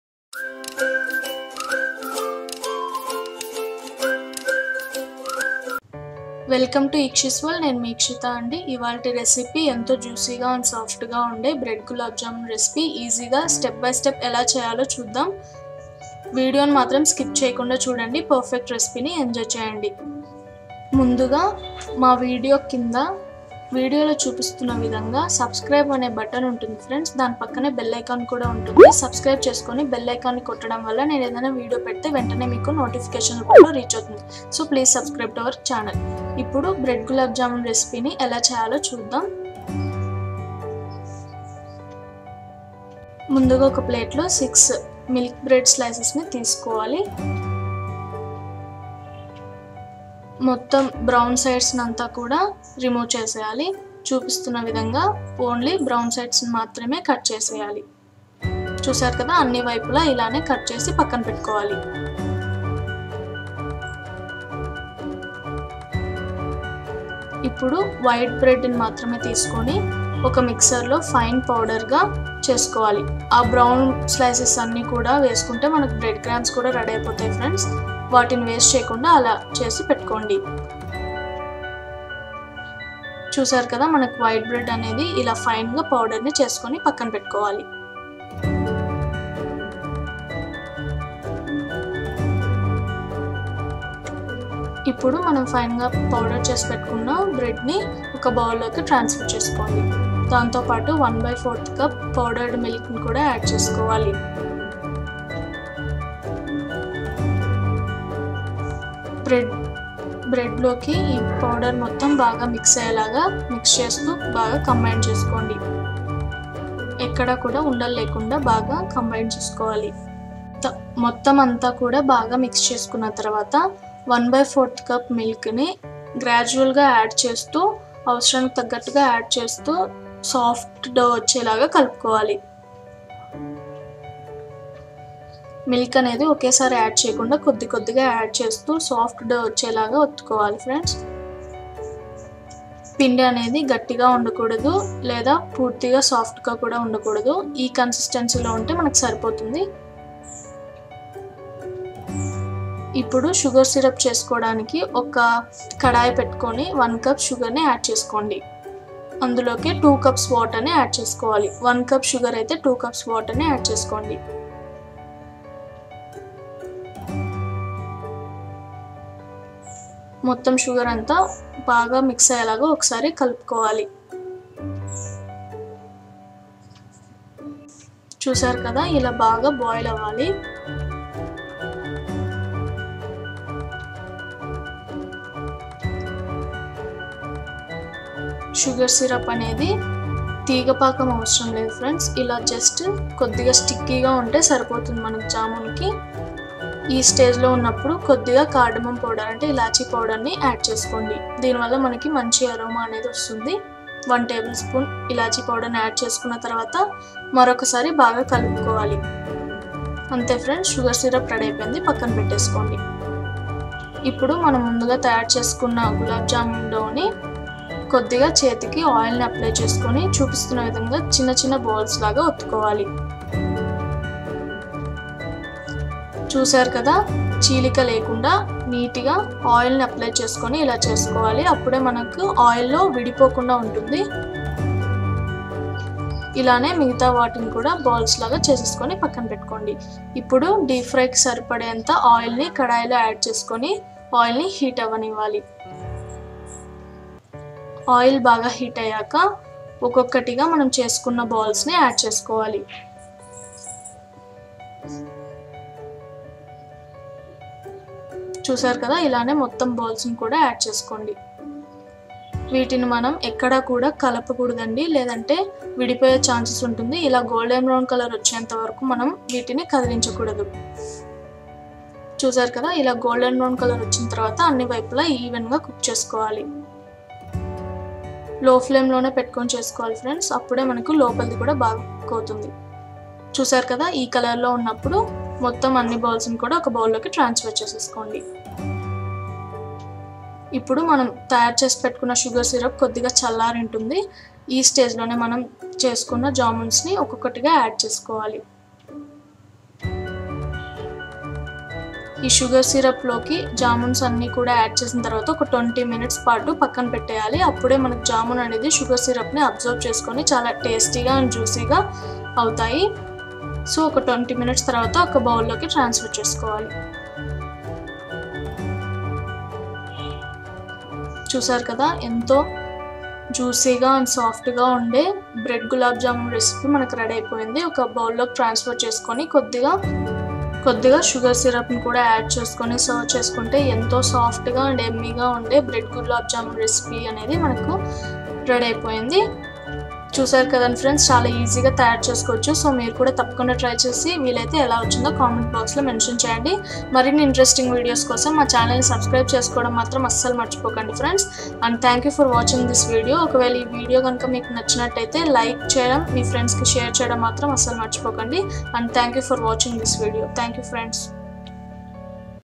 Soft And Juicy Bread Gulab Jamun | With Tips | బ్రెడ్ గులాబ్ జామూన్ | Telugu Channel| Eekshi's World
Soft And Juicy Bread Gulab Jamun | With Tips | బ్రెడ్ గులాబ్ జామూన్ | Teluguchannel Eekshi's World

INGREDIENTS:
MILK BREAD
MILK PWD
ELACHI
SUGAR
WATER
TURMERIC

PLEASE...
AND
AND DON'T FORGET TO CLICK ON BELL ICON..

THANK U..